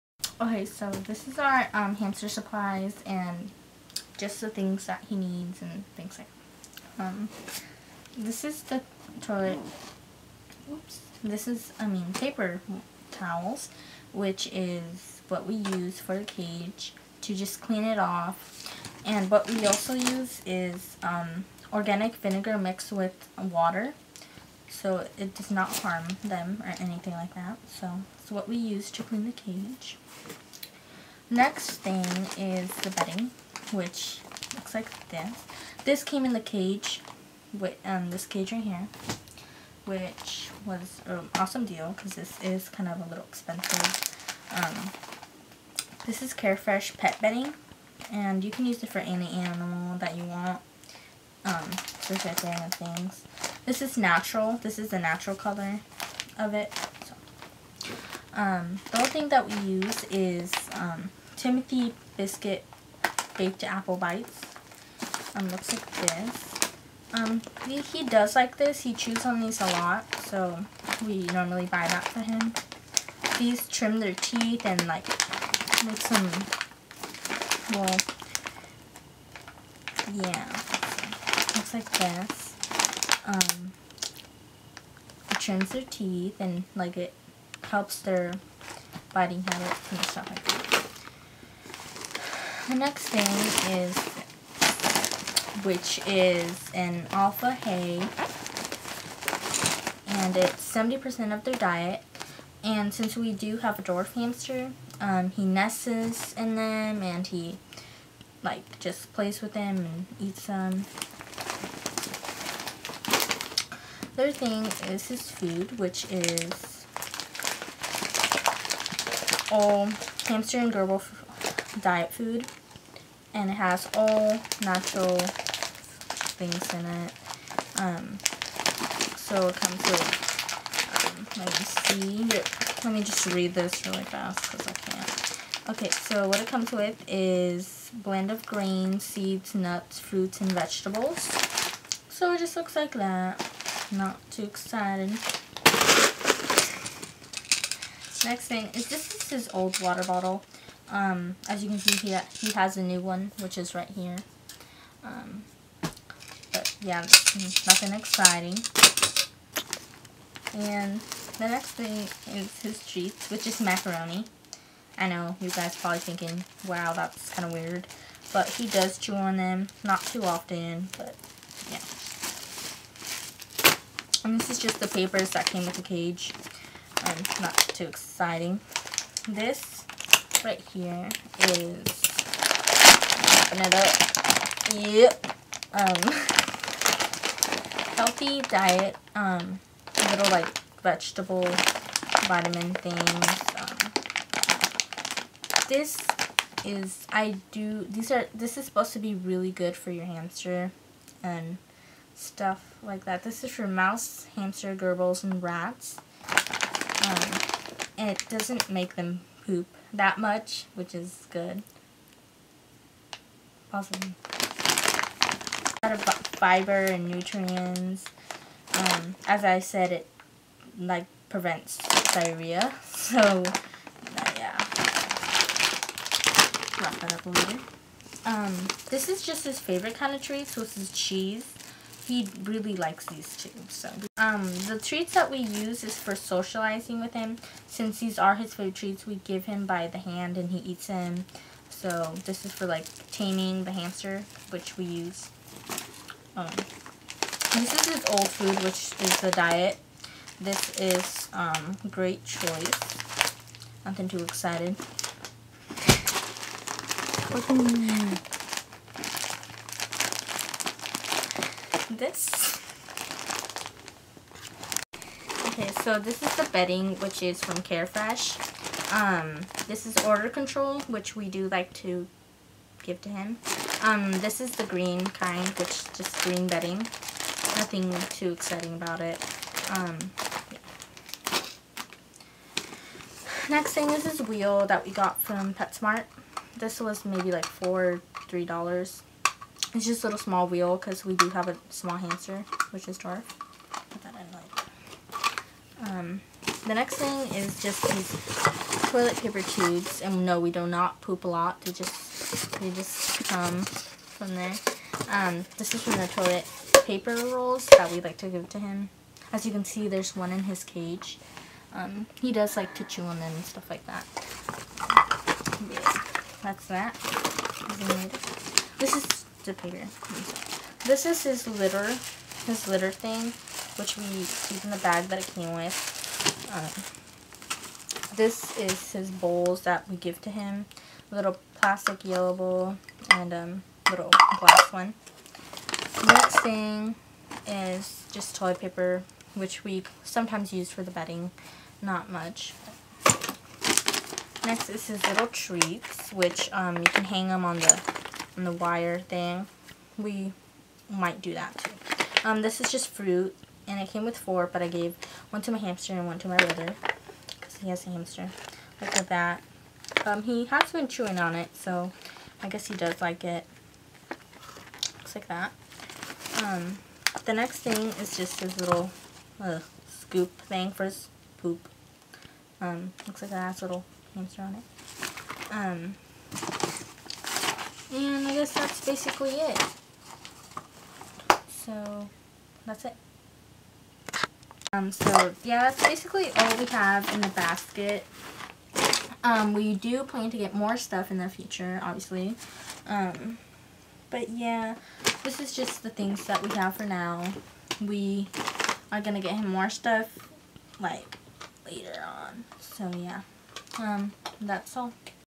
Hamster organization- supplies
We keep our hamster supplies and all his things in a basket and we're just showing whats in it and what we have. We still need a few more things and plan on getting more in the near future plus the bedding that runs out pretty quick.